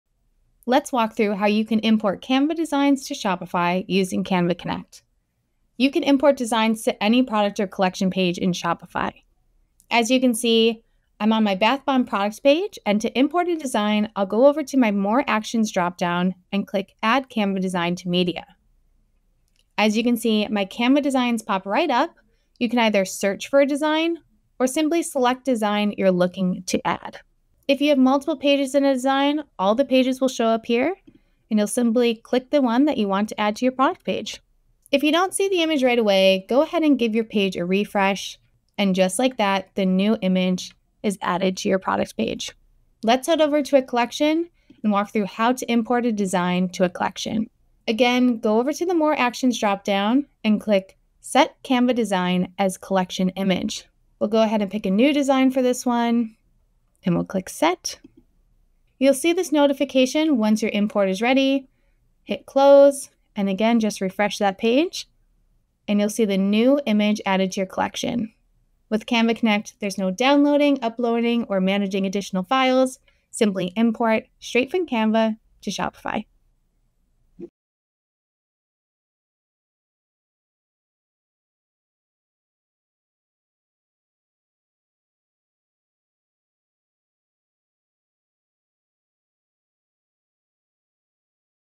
Shopify Connect for Canva | How to import a Canva design in Shopify
With Shopify Connect for Canva, you can retrieve your Canva designs without leaving Shopify and import them to any product or collection page.

Chapters:
0:12: Add Canva design to a product page
0:35: Refresh the page if you don’t see the design
0:43: Add Canva design to a collection

Check out the other videos in our Shopify Connect for Canva playlist for ways to leverage and enhance your Shopify product images.